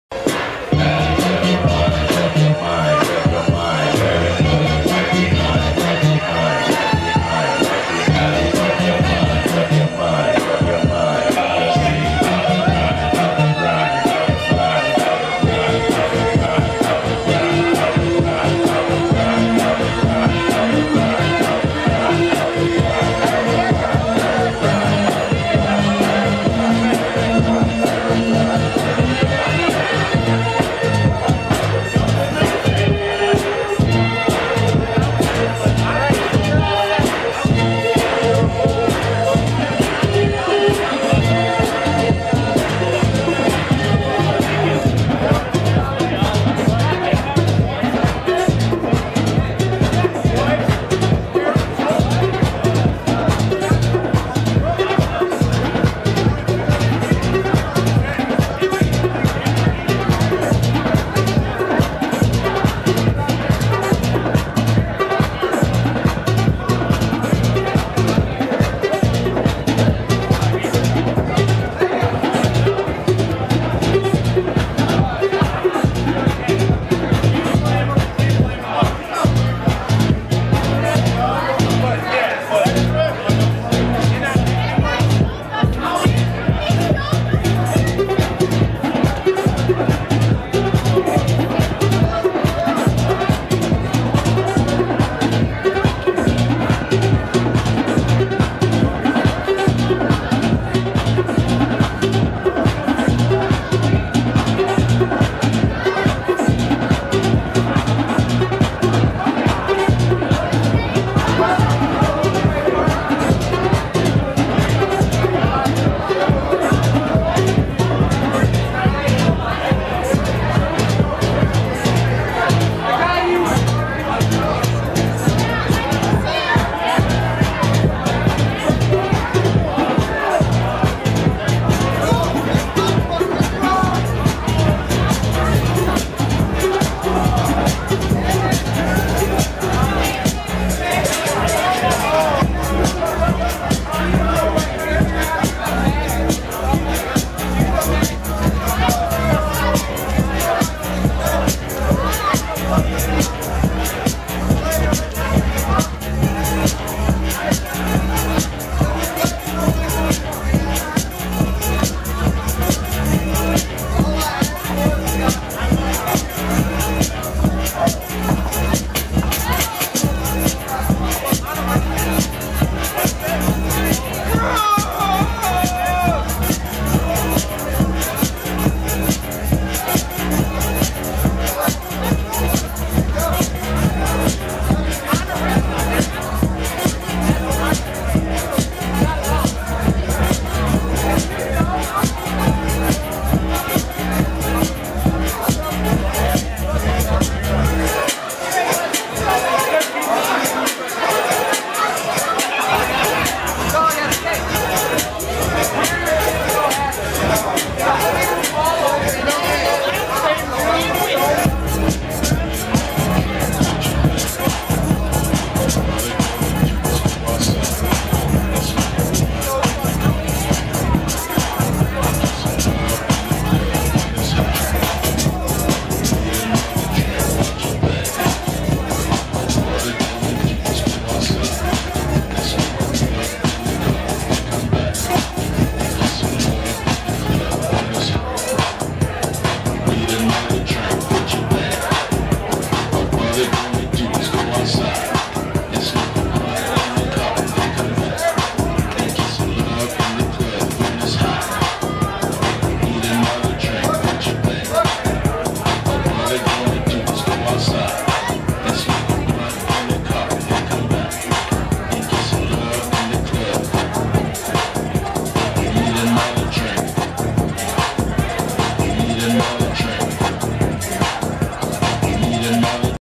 Brian Gillespie working it at The Works-Detroit Jan 26, 2008
Brian Gillespie opens the night in the front room at The Works on January 26, 2008.